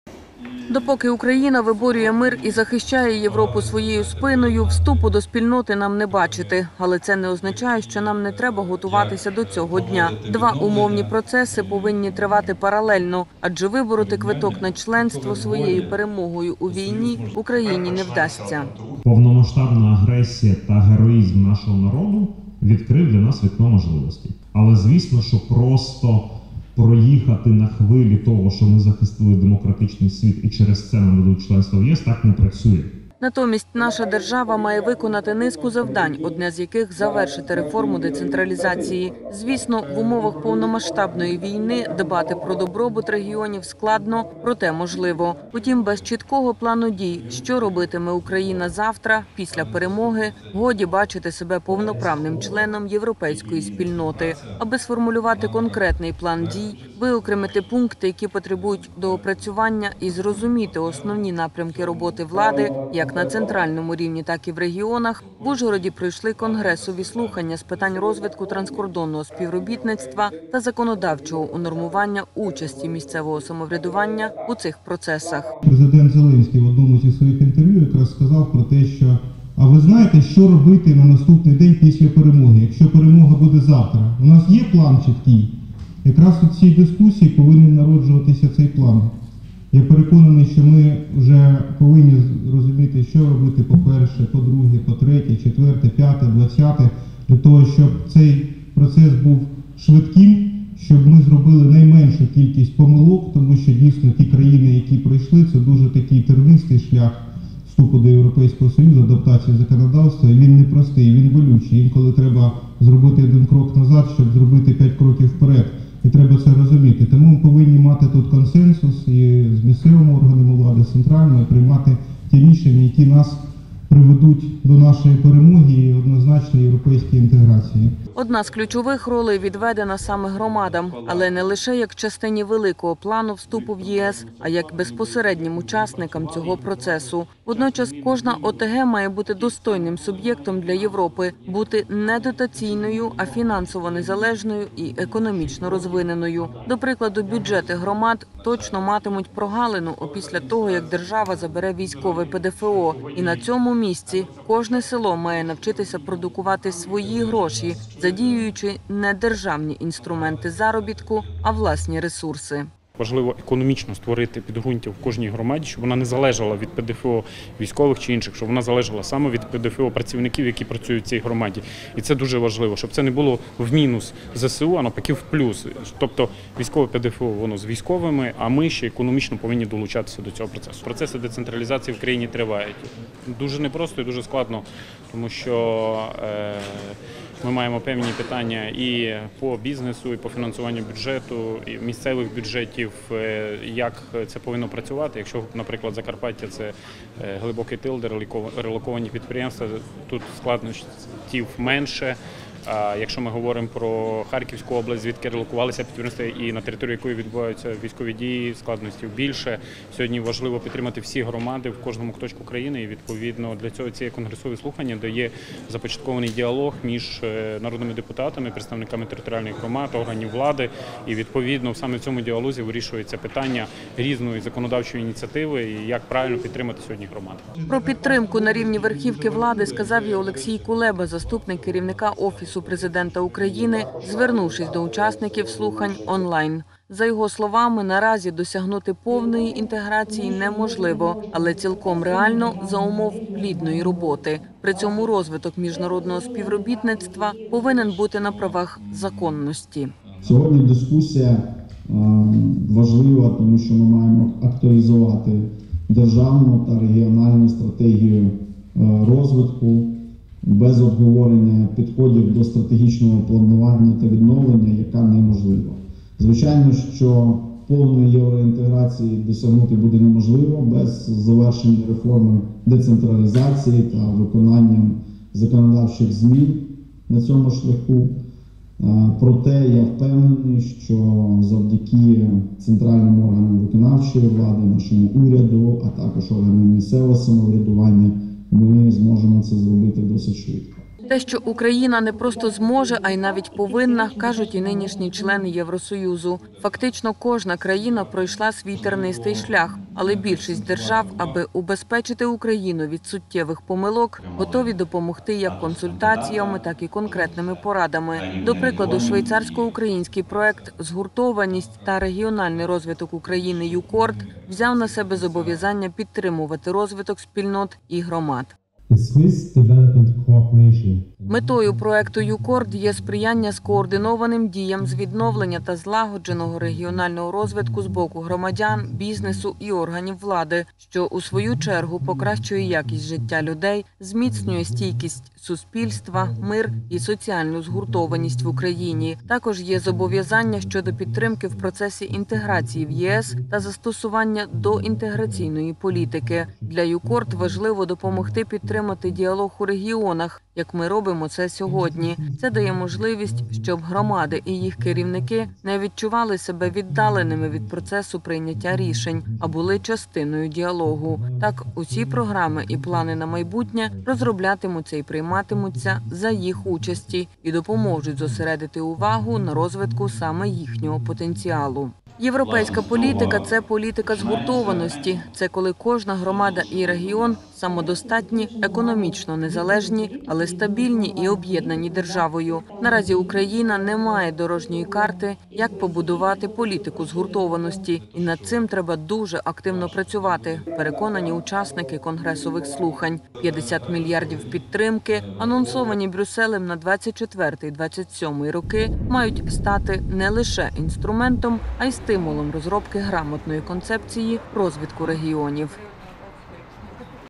В Ужгороді відбулися конгресові слухання
Допоки Україна виборює мир і захищає Європу своєю спиною, вступу до спільноти нам не бачити. Але, це не означає, що нам не треба готуватися до цього дня. Два умовні процеси повинні тривати паралельно, адже вибороти квиток на членство своєю перемогою у війні, Україні не вдасться.